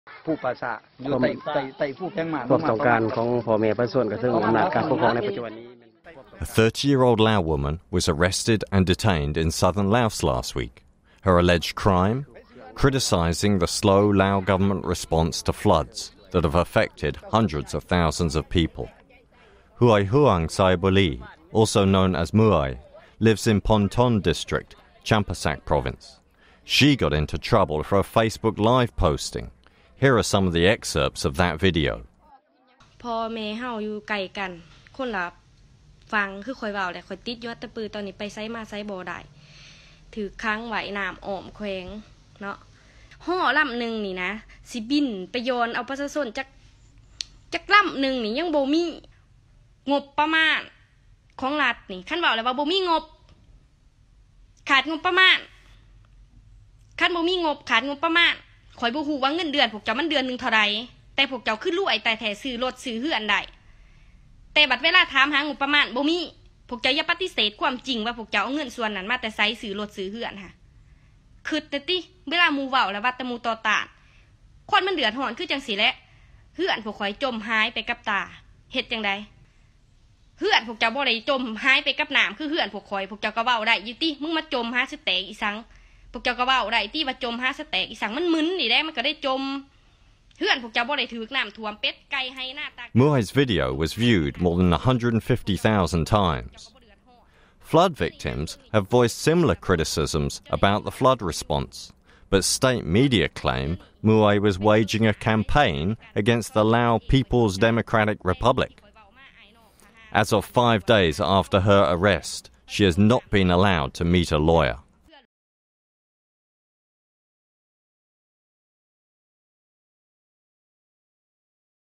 Lao Authorities Arrest Woman for Criticizing Flood Relief Efforts | Radio Free Asia (RFA)
A woman in Laos was arrested last week on charges of defaming the country after she posted criticism on Facebook of the slow government response to severe floods in the country’s southern provinces, sources say.